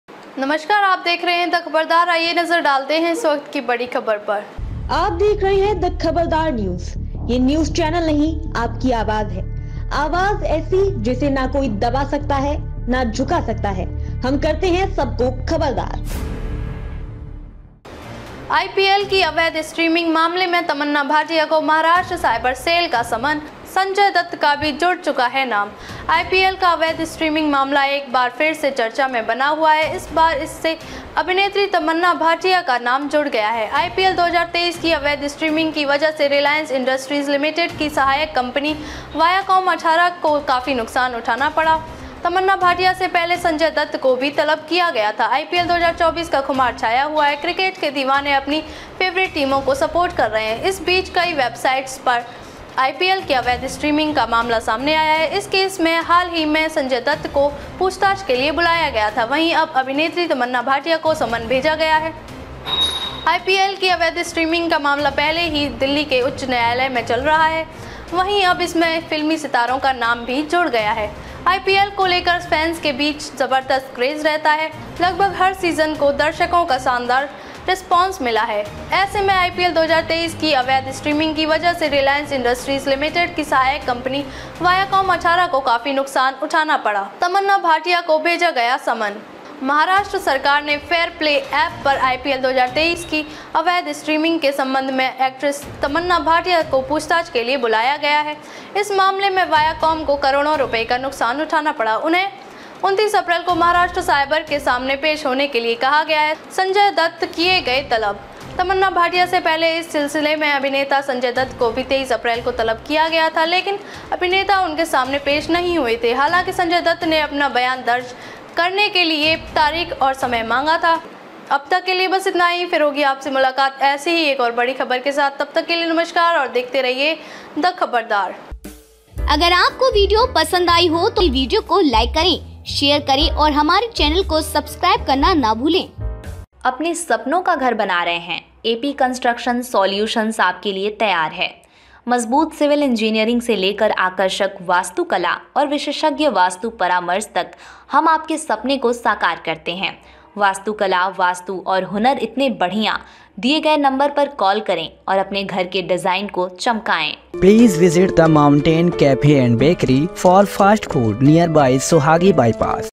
IPL की अवैध स्ट्रीमिंग मामले में Tamannaah Bhatia को Maharashtra साइबर सेल का समन, Sanjay Dutt का...
आईपीएल का अवैध स्ट्रीमिंग मामला अब एक बार फिर चर्चा में बना हुआ है। इस बार इससे अभिनेत्री तमन्ना भाटिया (Tamannaah Bhatia) का नाम जुड़ गया है। आईपीएल 2023 (IPL 2023) की अवैध स्ट्रीमिंग की वजह से रिलायंस इंडस्ट्रीज लिमिटेड (IRL) की सहायक कंपनी वायाकॉम 18 को काफी नुकसान उठाना पड़ा। तमन्ना भाटिया से पहले संजय दत्त को भी तलब किया गया था।